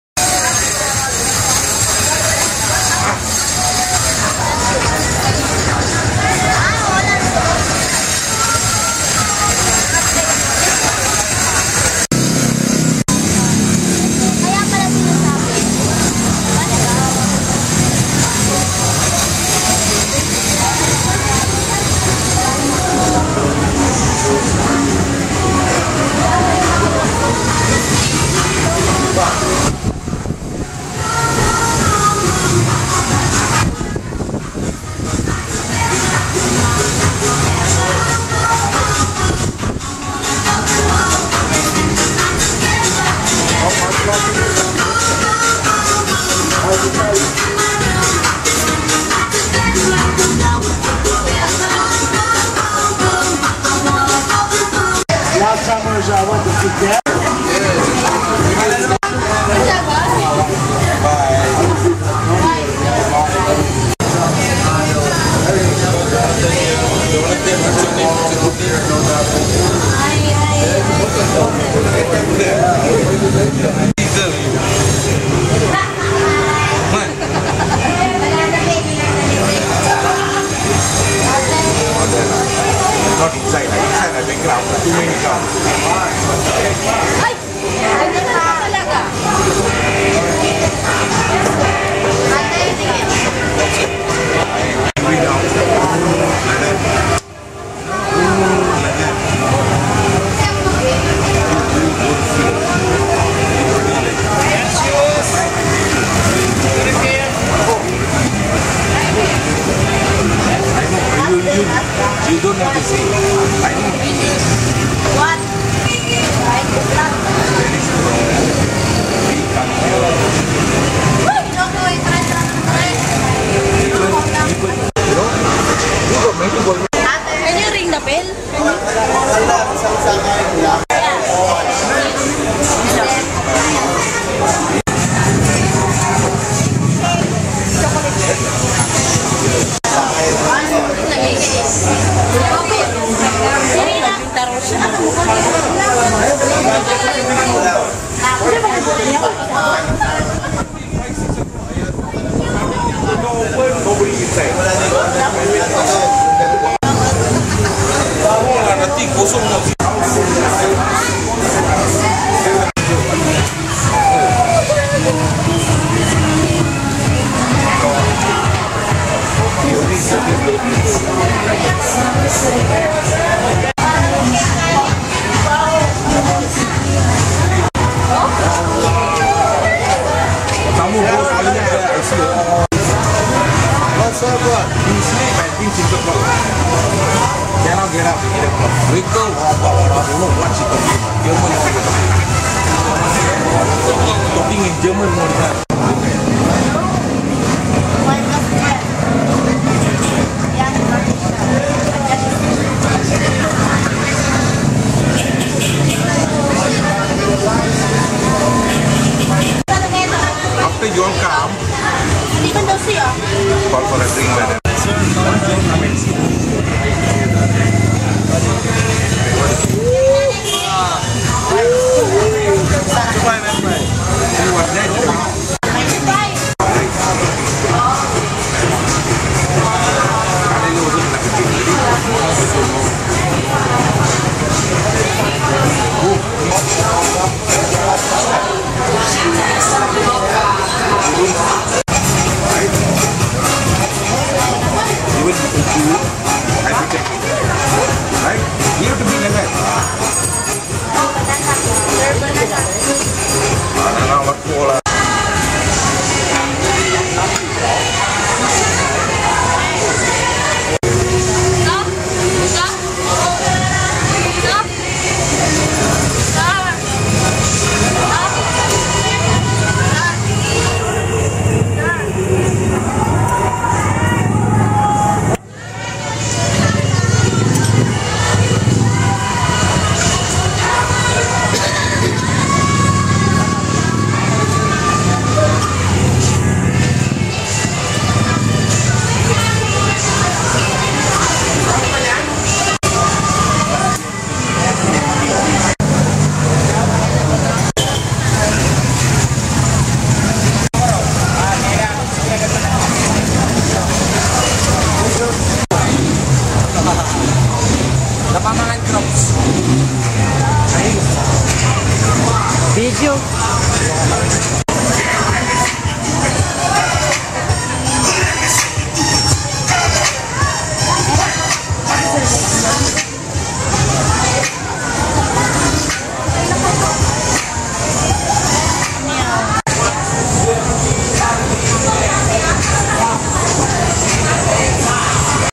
Kokomo's, Fields Avenue, Angeles City
A Fields Avenue institution. Bar and restaurant right in the middle of the action, open 24 hours 7 days a week. A prime vantage point for people watching.